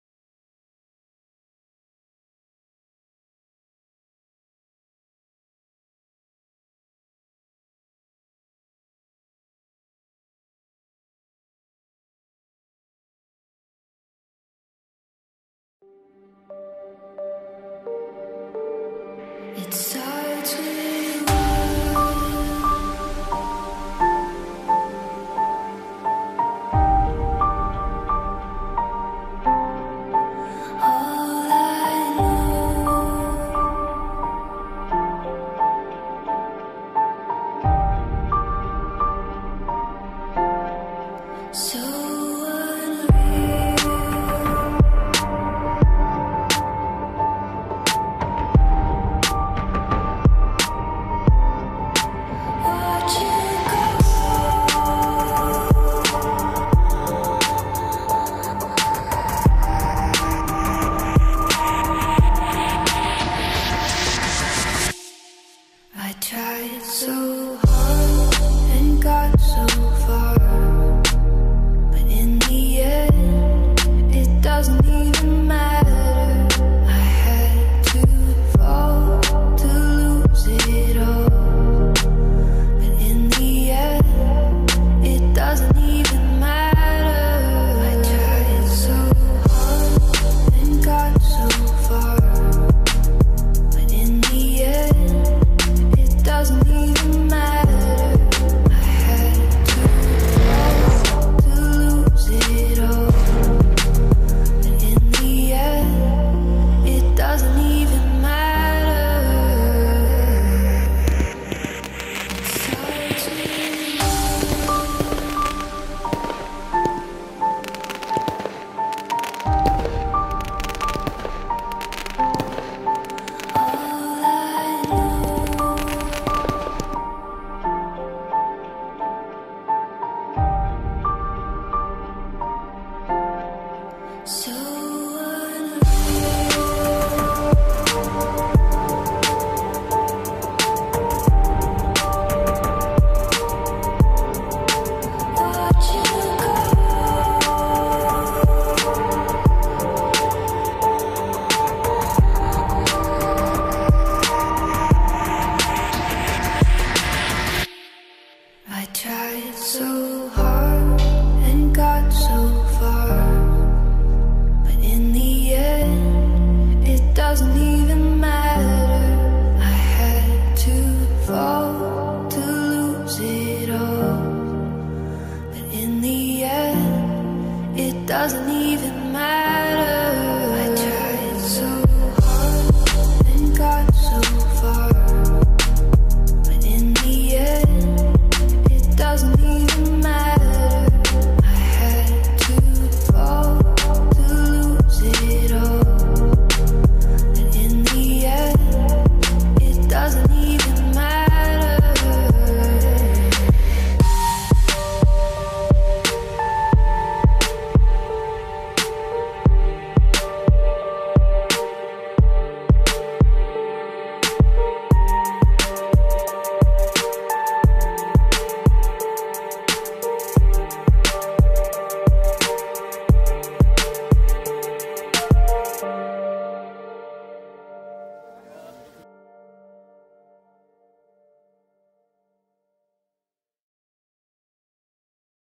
Saisonrückblick Herbst 2020
Eine Abnormale Herbstsaison. Am Anfang mit wenig Einschränkung, doch zum Ende hin wieder mehr. Das letzte Spiel wurde leider komplett abgesagt. Der FCA steht allerdings auf Platz 1 und spielt wahnsinnig gut oben mit. Auch wir konnten mit einigen Choreos, Pyros, Spruchbänder und Aktionen für Furore, Entsetzung und Staunen sorgen.